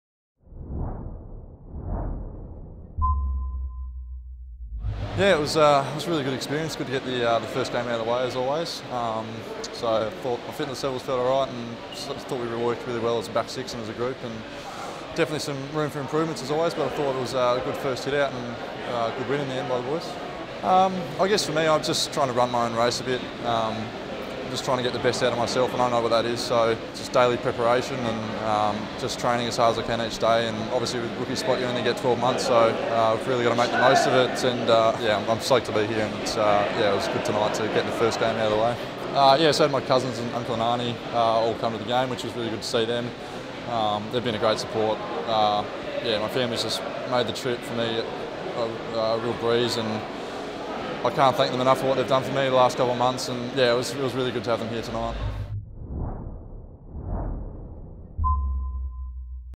Schade's moment in the sun
Hear from new Magpie Henry Schade after his first appearance in the Black and White against Essendon in the JLT Community Series opener.